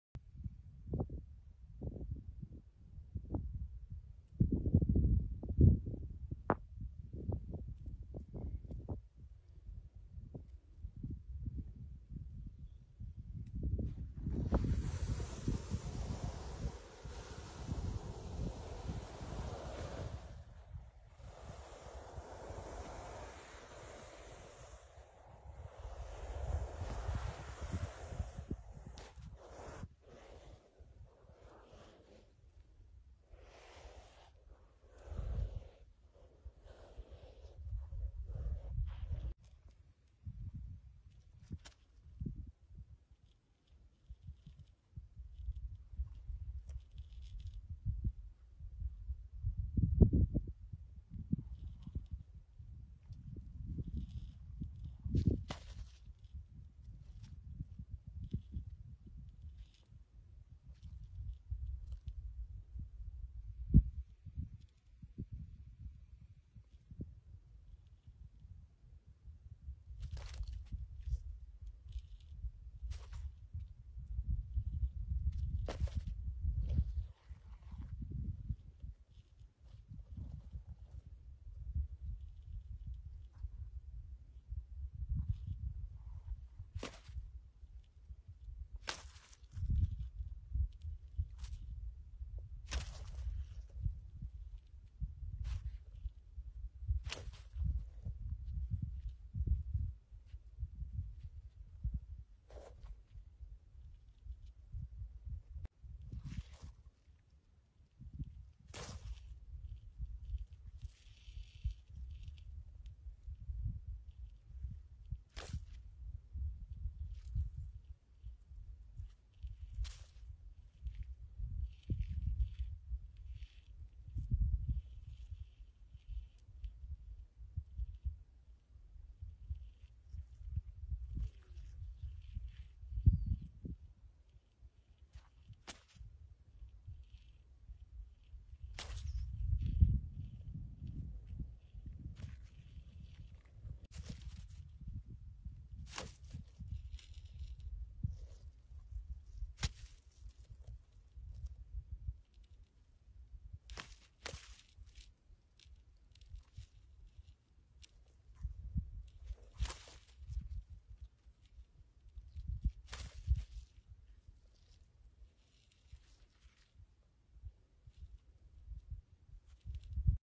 Sand Cat Vs Saharan horned viper Snake.
Cerastes cerastes, commonly known as the Saharan horned viper or the desert horned viper, is a venomous species of viper native to the deserts of Northern Africa and parts of the Arabian Peninsula and Levant. It is often easily recognized by the presence of a pair of supraocular "horns", although hornless individuals do occur. Three subspecies have been described.